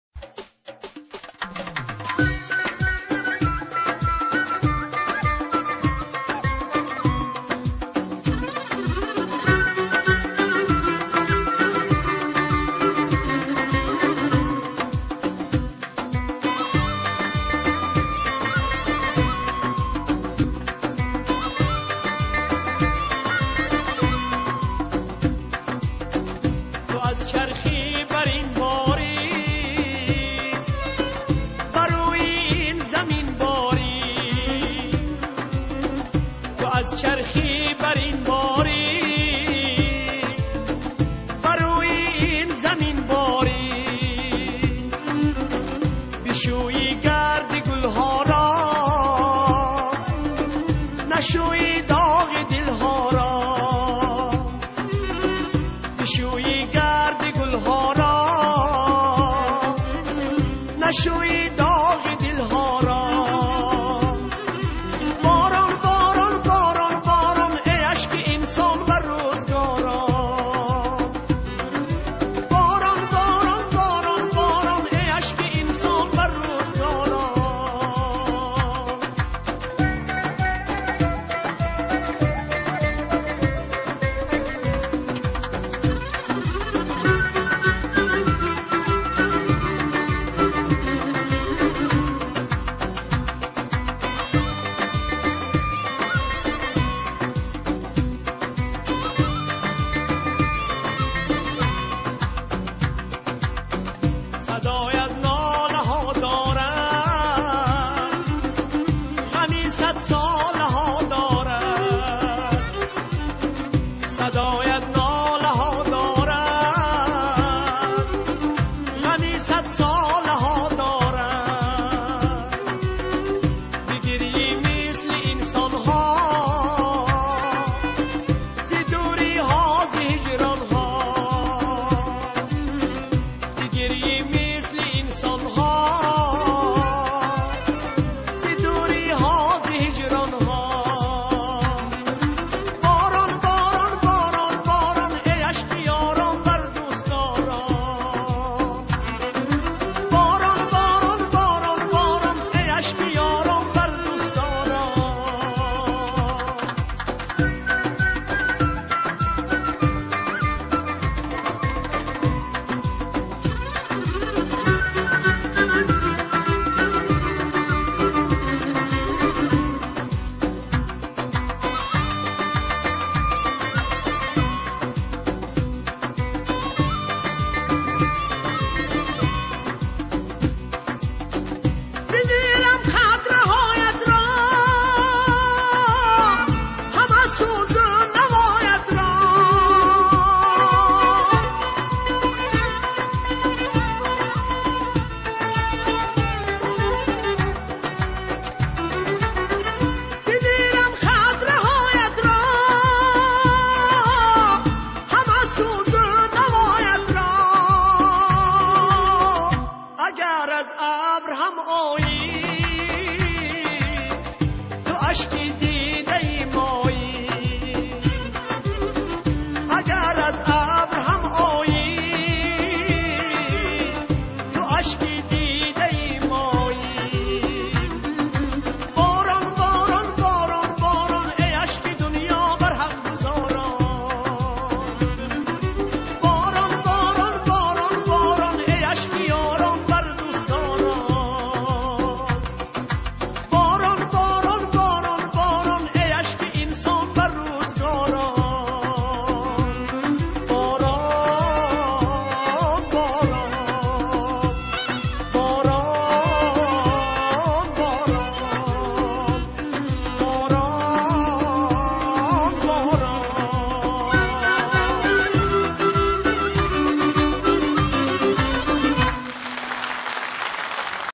Khosiyati Zarafshoni - Live In-Concert | Boron
Khosiyati Zarafshoni (aka Ortiqova), the People's artist of Tajikistan, was born in 1960 in Aini District, Viloyati Sughd, Tajikistan.

Upon completion of Arts Institute in Tajkistan, in 1982, she embarks upon her lengthy and successful career of a singer. Her mentor is Jurabek Murodov. This concert is dedicated to her 25 years in creative scene.